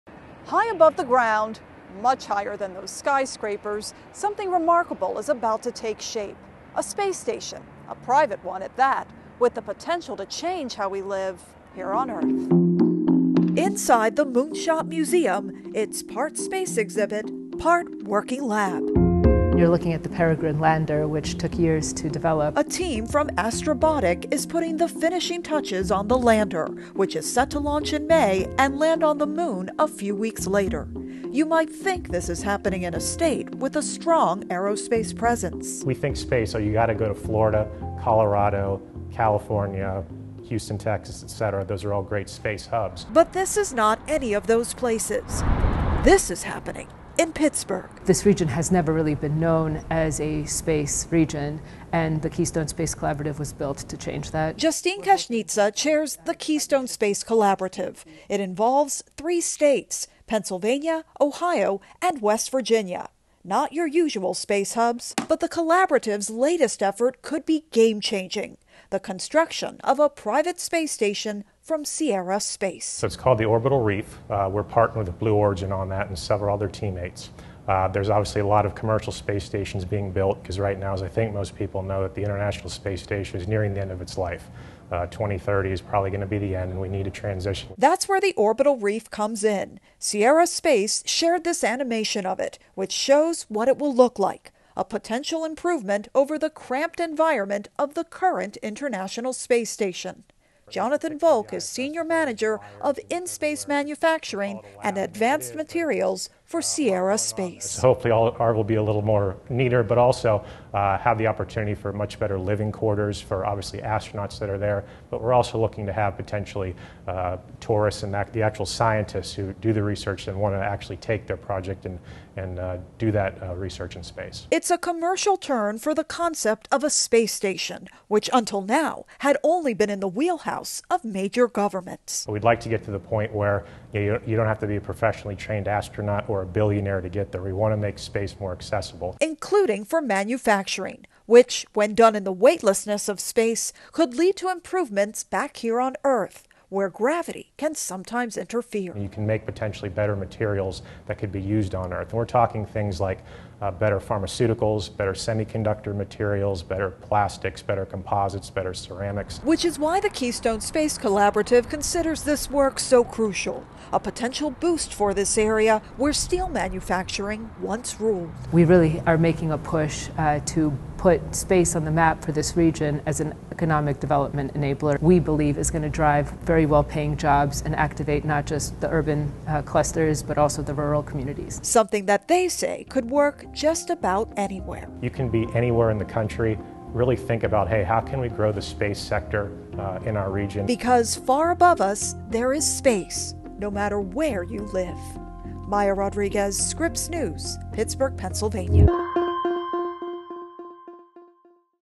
Private space stations take shape amid new space exploration era
"We think space — 'Oh, you've got to go to Florida; Colorado; California; Houston, Texas; etc.," said Jonathan Volk of Sierra Space. "Those are all great space hubs." However, we're not speaking of those places. Scripps News' Maya Rodriguez has more!